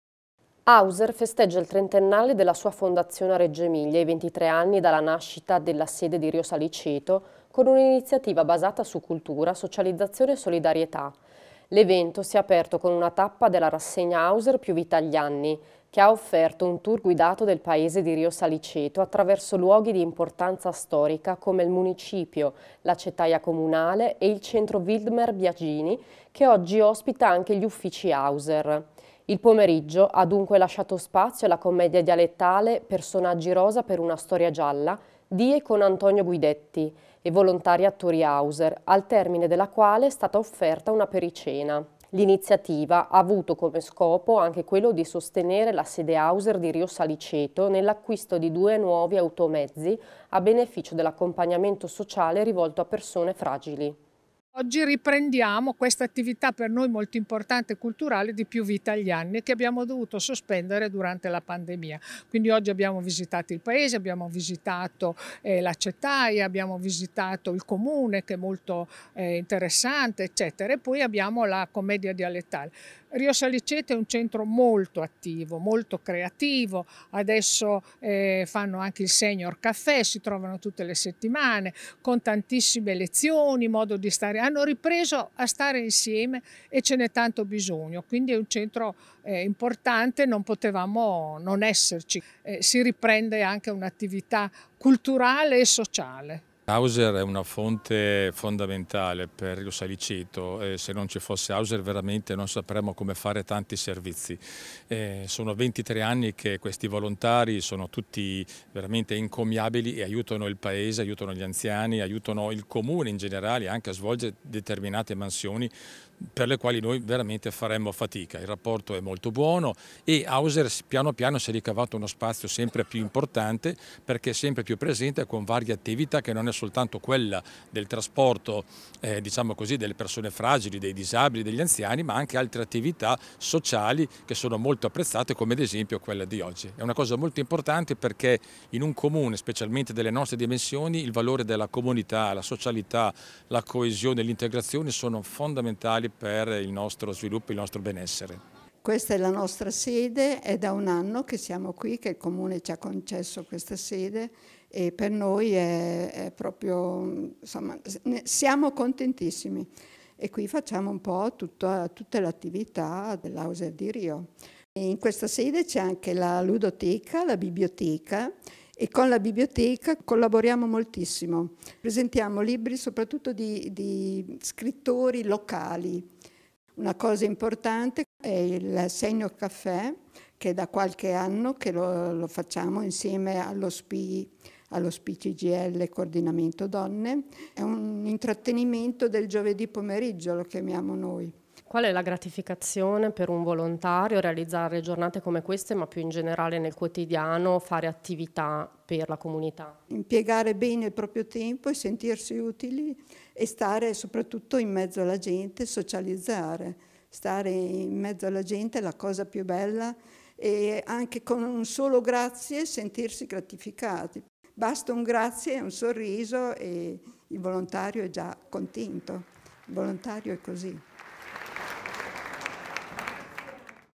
Auser Rio Saliceto festeggia il compleanno dell'associazione
Auser festeggia il trentennale della sua fondazione a Reggio Emilia e i 23 anni dalla nascita del centro di Rio Saliceto con una iniziativa basata su cultura, socializzazione e solidarietà. L’evento si è aperto con una tappa della rassegna Auser Più vita agli anni che ha offerto un tour guidato del paese di Rio Saliceto, attraverso luoghi di importanza storica come il Municipio, l’acetaia comunale e il museo Wildmer Biagini, che oggi ospita anche gli uffici Auser. Il pomeriggio ha, dunque, lasciato spazio alla commedia dialettale Personaggi rosa per una storia gialla, di e con Antonio Guidetti e volontari-attori Auser, al termine della quale è stata offerta un’apericena.
L’iniziativa ha avuto come scopo anche quello di sostenere la sede Auser di Rio Saliceto nell’acquisto di due nuovi automezzi a beneficio dell’accompagnamento sociale rivolto a persone fragili.

Servizio di Lucia Cuccurese. Riprese di Massimo Manini.